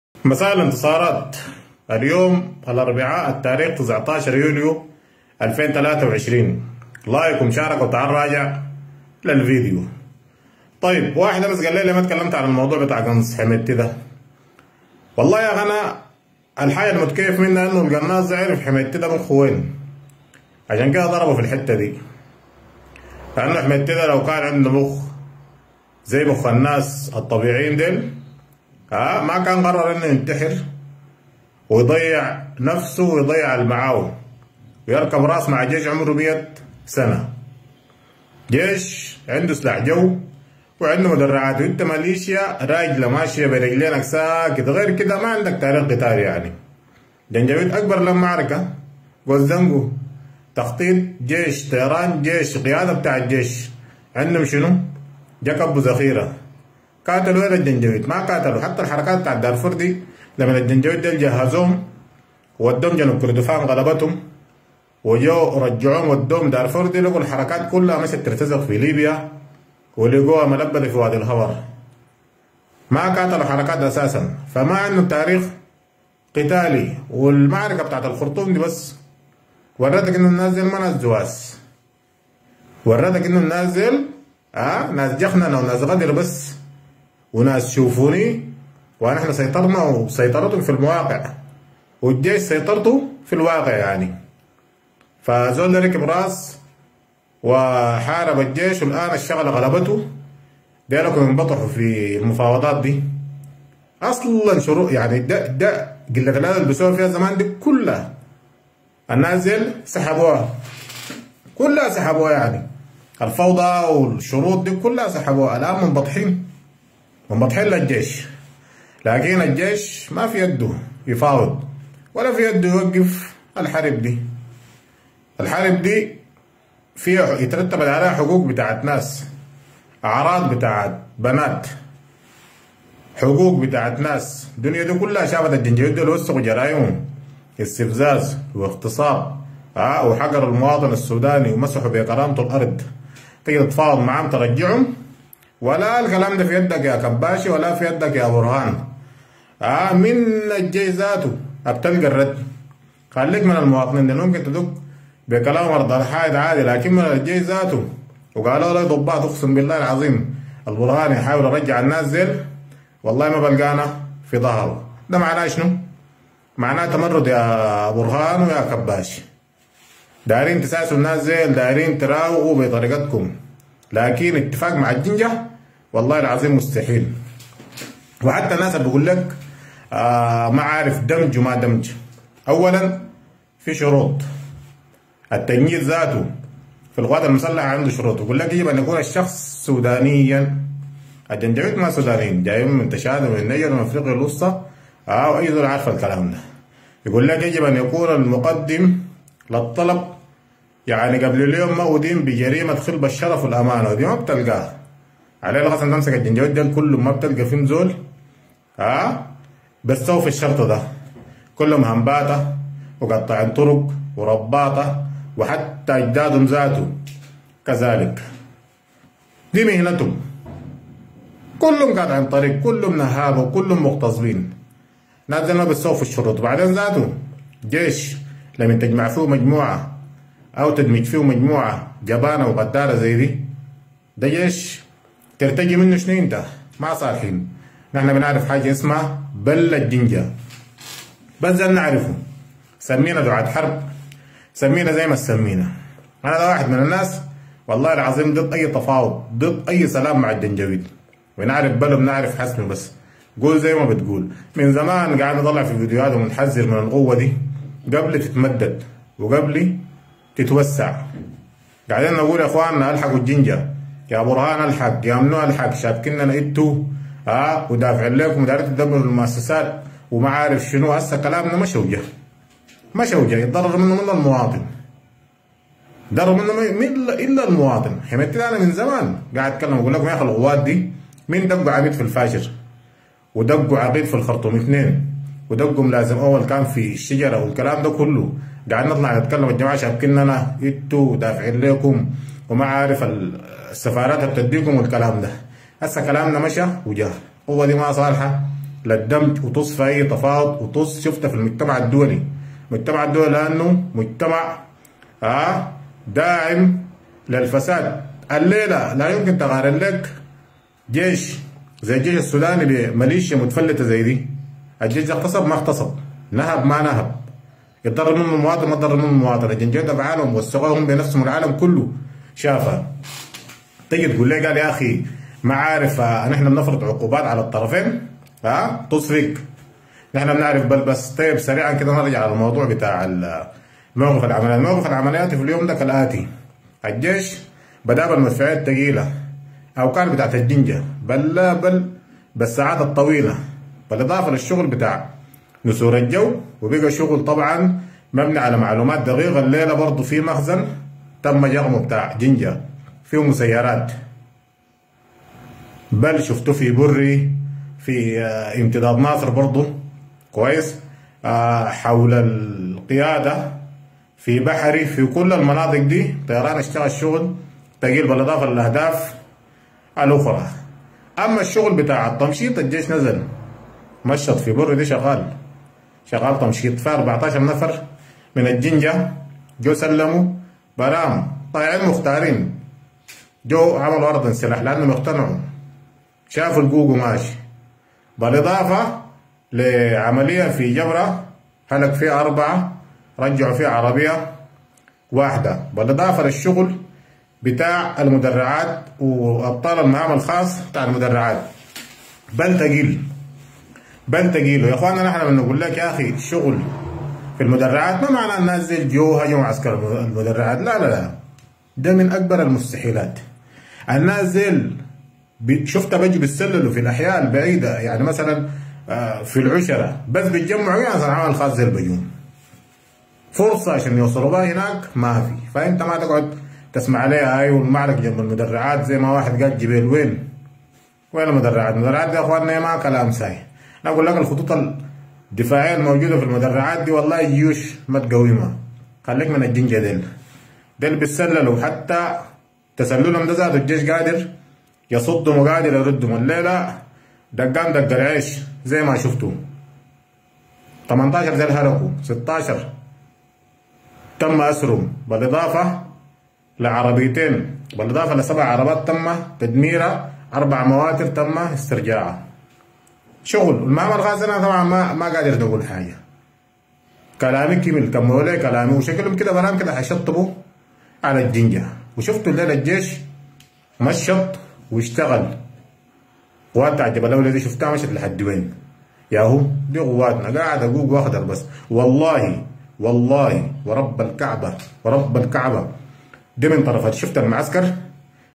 الشرطي ابراهيم | الى البرهان وكباشي ماتبقى من المليشيا غيرصالح للدمج مساء اليوم الاربعاء 19-07-2023م
الشرطي ابراهيم | البرهان يتعرض لضغط.القناص عرف اين يوجد عقل حميدتي بحري ام درمان الخرطوم وباقي المناطق-الى البرهان وكباشي وباقي القيادات العليا ما تبقى من المليشيا غير صالح للدمج في الجيش مساء اليوم الاربعاء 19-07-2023م